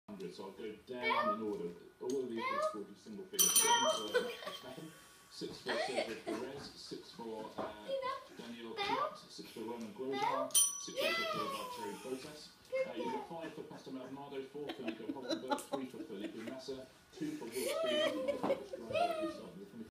LUNA'S FUNNY MOMENTS | BELL RINGING
Here's Luna ringing her bell!